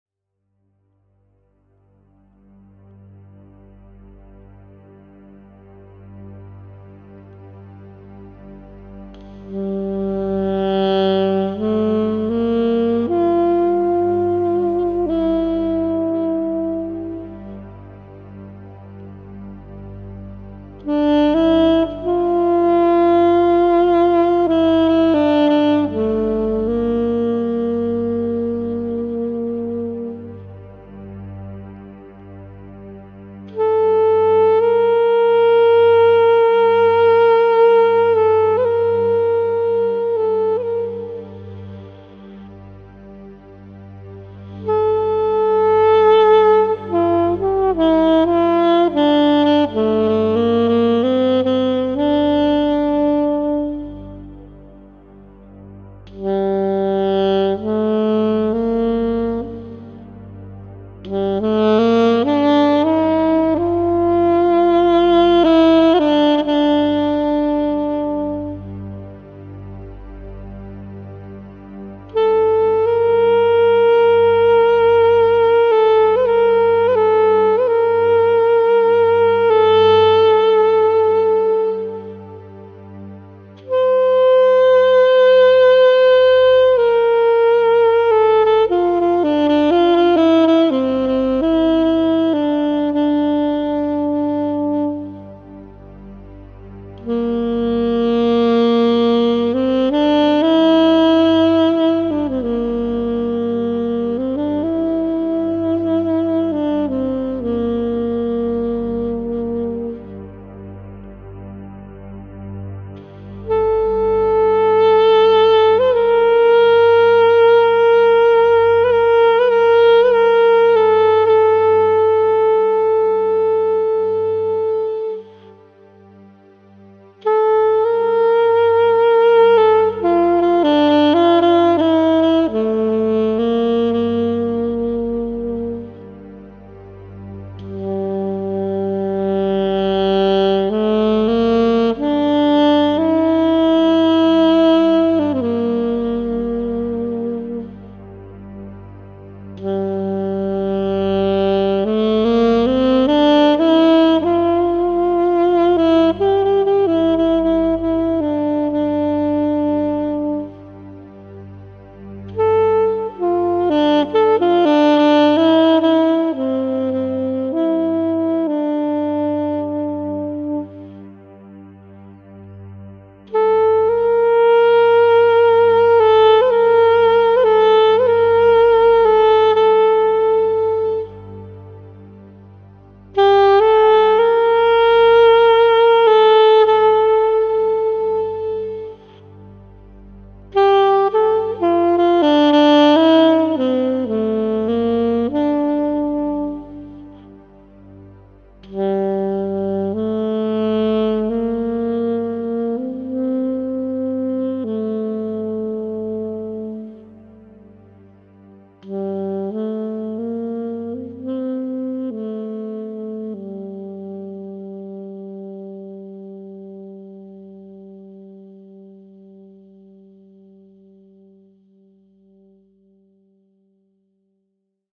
Flatsax @ Ewen Maddock Dam_16March'23
Sunrise Session 27 - I'm playing my 'Flatsax' (made from plywood and  2 lengths of 12mm x 12mm pine and a soprano sax mouthpiece). The instrument is pitched like an alto sax (it's a tone higher, in F). It plays nearly 2 octaves (f - e'') and is chromatic, using cross fingering similar to a recorder. This session was a sunrise improvisation recorded on the edge of Ewen Maddock Dam, near Landsborough, Qld. Australia.